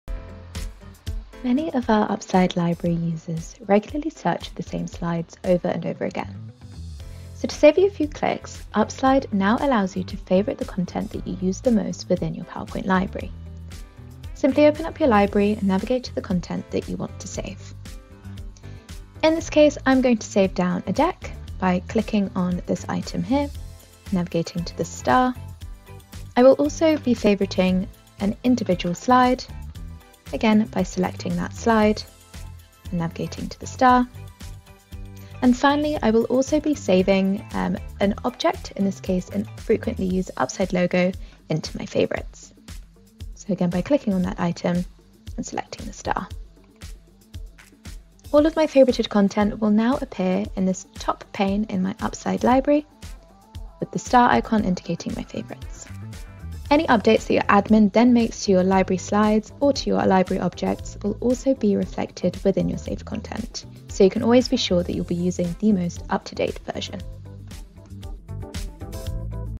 Save your favourite Library items in one click
Ever wanted to save your favourite hashtag#PowerPoint slides in-app for your own quick access?💡

With our latest update to your UpSlide ribbon, you're now able to build decks even more efficiently.

Watch Kimya Haghighat, Consultant at UpSlide, as she walks us through how to use the Library Favourites feature to optimise your workflows in PowerPoint.

For more productivity tips and tricks in PowerPoint, Excel, and Word, sign up to our newsletter 📨

hashtag#NewFeatures hashtag#Productivity hashtag#Finance